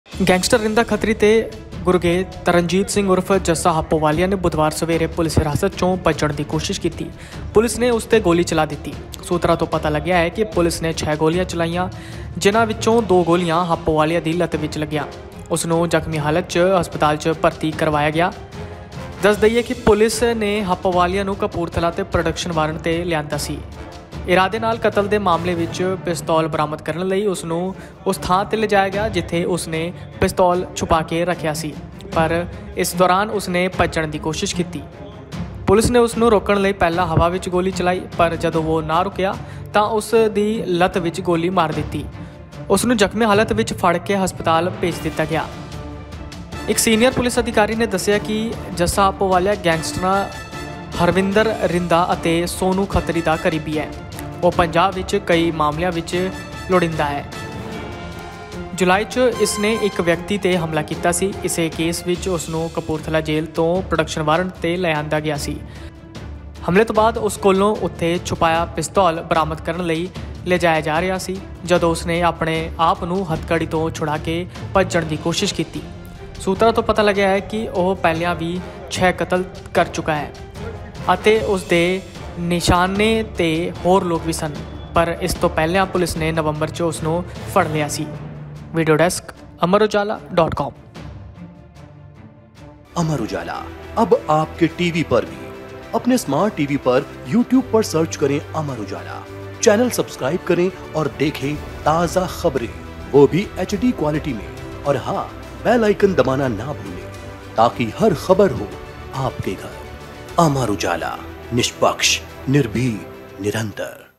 Jassa happowal Encounter: ਰਿੰਦਾ-ਖੱਤਰੀ ਦਾ ਗੁੰਡਾ ਪੁਲਿਸ ਮੁਕਾਬਲੇ ਦੌਰਾਨ ਫਰਾਰ ਹੋਣ ਦੀ ਕੋਸ਼ਿਸ਼ ਦੌਰਾਨ ਜ਼ਖਮੀ
ਗੈਂਗਸਟਰ ਰਿੰਦਾ-ਖਤਰੀ ਦੇ ਸਰਗਨਾ ਤਰਨਜੀਤ ਸਿੰਘ ਉਰਫ ਜੱਸਾ ਹਾਪੋਵਾਲੀਆ ਨੇ ਬੁੱਧਵਾਰ ਸਵੇਰੇ ਪੁਲਸ ਹਿਰਾਸਤ 'ਚੋਂ ਭੱਜਣ ਦੀ ਕੋਸ਼ਿਸ਼ ਕੀਤੀ। ਪੁਲਿਸ ਨੇ ਉਸ 'ਤੇ ਗੋਲੀ ਚਲਾ ਦਿੱਤੀ। ਸੂਤਰਾਂ ਤੋਂ ਪਤਾ ਲੱਗਾ ਹੈ ਕਿ ਪੁਲੀਸ ਨੇ ਛੇ ਗੋਲੀਆਂ ਚਲਾਈਆਂ, ਜਿਨ੍ਹਾਂ ਵਿੱਚੋਂ ਦੋ ਗੋਲੀਆਂ ਹਾਪੋਵਾਲੀਆ ਦੀ ਲੱਤ ਵਿੱਚ ਲੱਗੀਆਂ। ਉਸ ਨੂੰ ਜ਼ਖਮੀ ਹਾਲਤ 'ਚ ਹਸਪਤਾਲ 'ਚ ਭਰਤੀ ਕਰਵਾਇਆ ਗਿਆ ਹੈ।